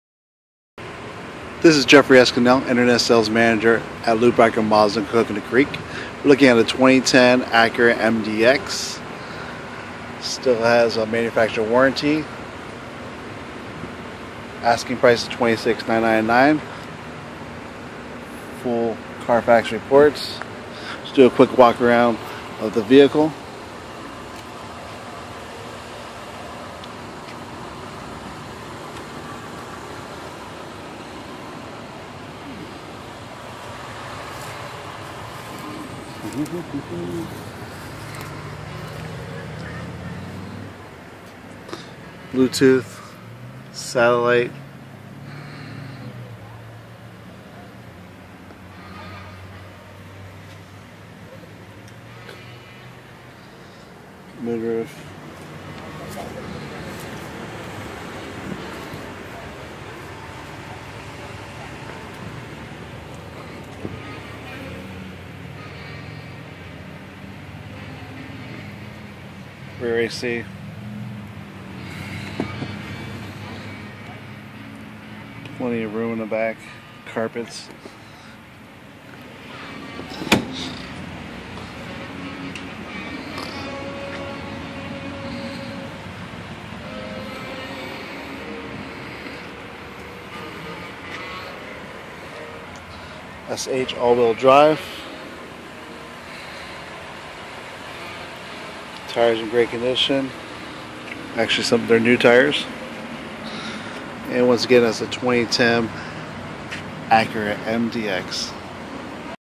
2010 Acura MDX at Lou Bachrodt
T his 2010 Acura MDX crystal Black Pearl with 33,000 plus miles 3.7 liter engine for $26995.